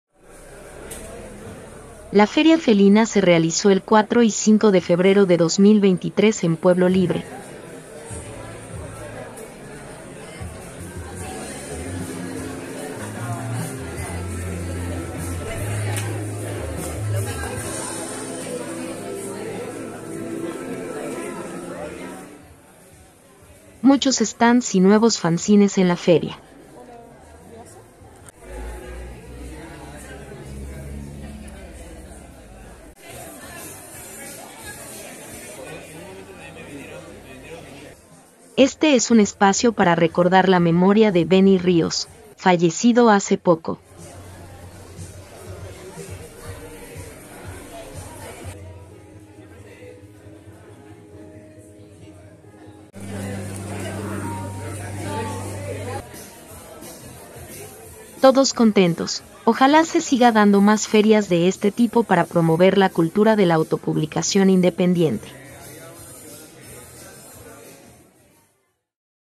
Feria Felina - Fanzines y más - Febrero de 2023
La feria felina se realizó entre el 4 y 5 de febrero de 2023 en el Galpón Espacio de Pueblo Libre. Feriantes presentaron fanzines, auto publicaciones,  diseño independiente y más.